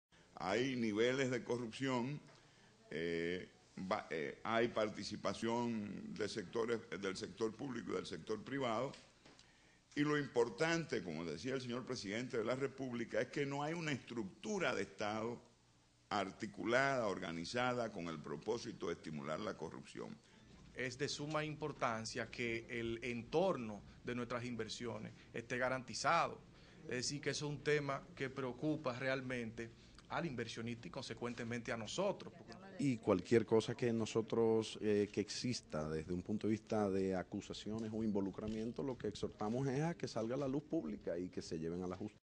Reaccionan en Dominicana ante informe de Estados Unidos sobre corrupcion
Religiosos, funcionarios y empresarios reaccionaron hoy ante el informe del gobierno de Estados Unidos que calificó como endémica la corrupción en el país.